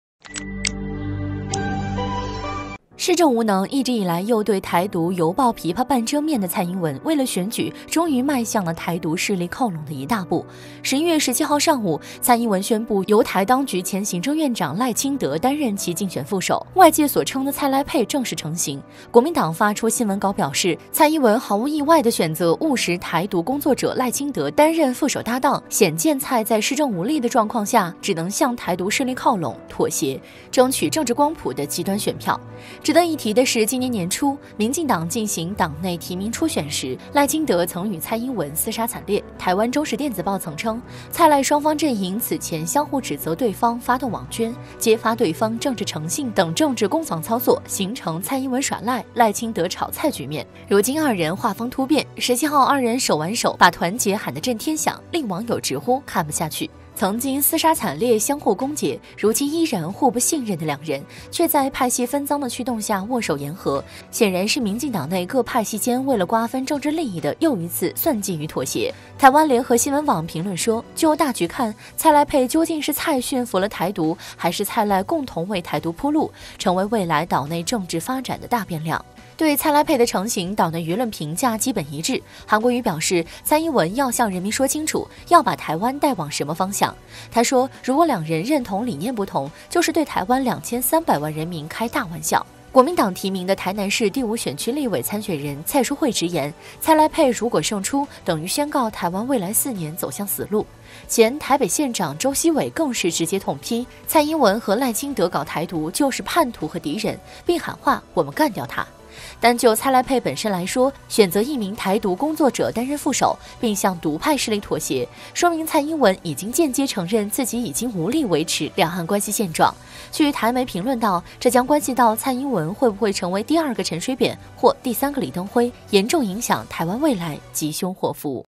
岛内舆论一致：蔡英文为大位向“极独”靠拢？选“独派”当副手
2019年11月20日